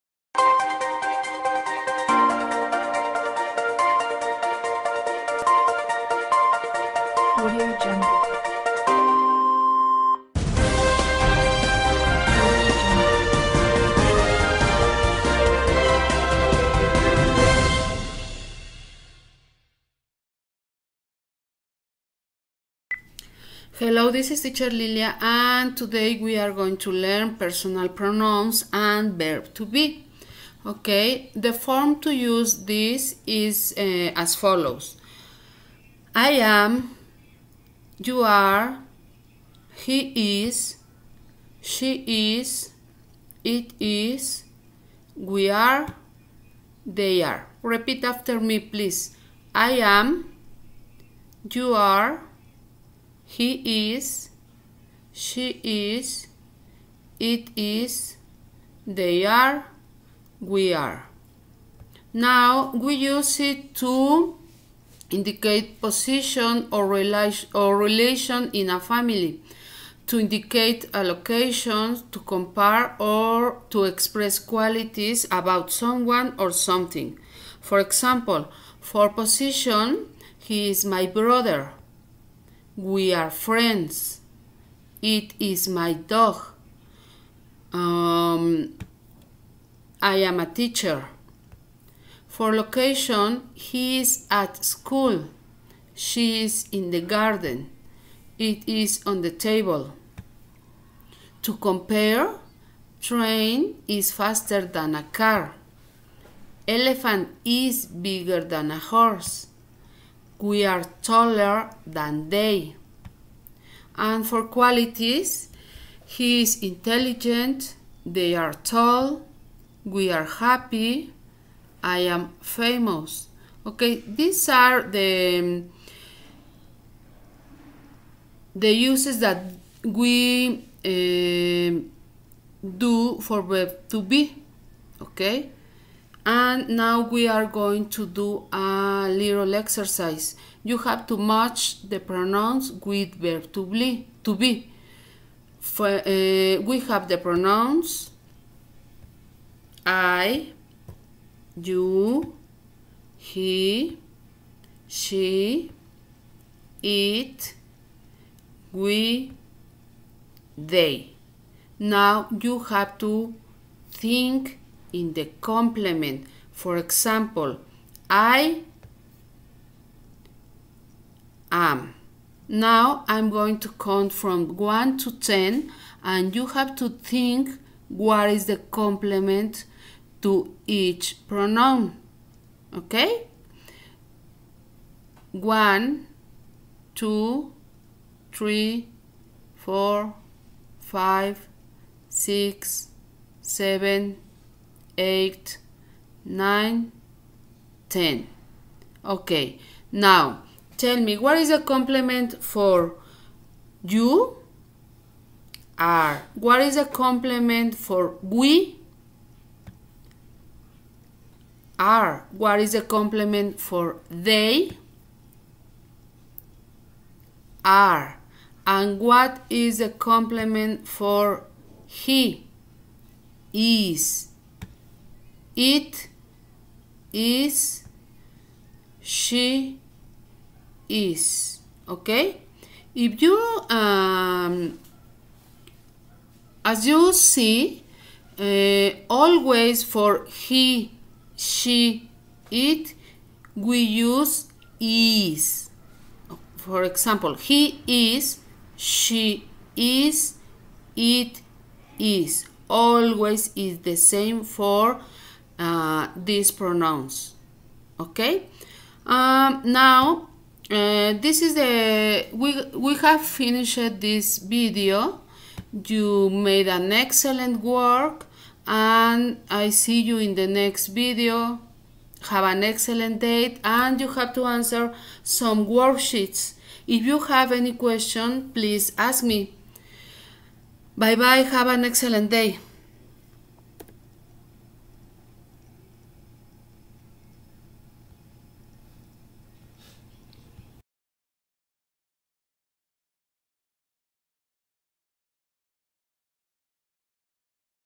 CLASE DE INGLES BÁSICO PRIMARIA BAJA - MIÉRCOLES 22 DE ABRIL DEL 2020
CLASE DE INGLES BÁSICO PRIMARIA BAJA, PERSONAL PRONOUNS AND VERB  TO BE.